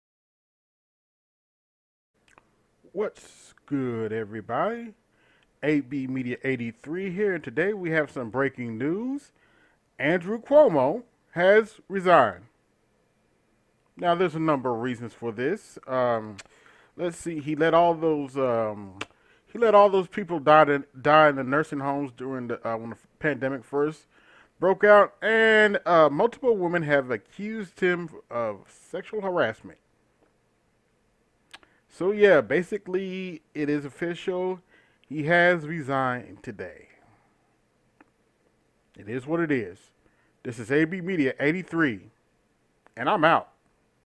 Breaking News!!!: Andrew Cuomo QUITS!!!
Breaking News
Andrew Cuomo has Resigned!

He's OUT!!!

Video Produced By Albert M. Byrd

Written and Directed by Albert M. Byrd